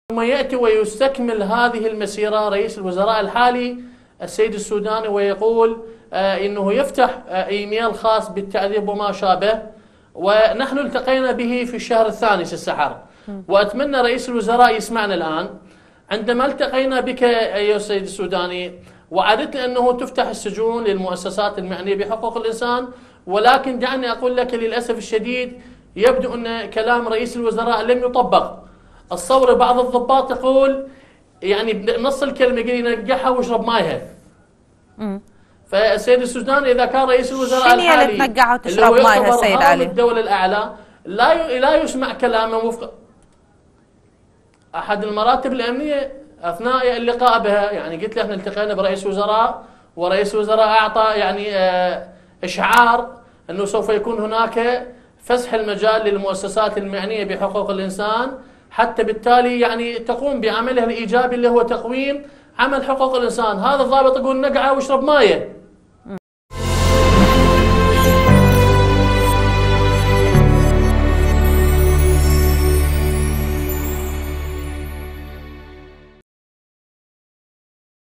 العبادي: السوداني وعدنا بفتح السجون امامنا واحد المراتب الامنية اخبرنا: نكع القرار واشرب ميه
12034/27500
H

11258📺 افقي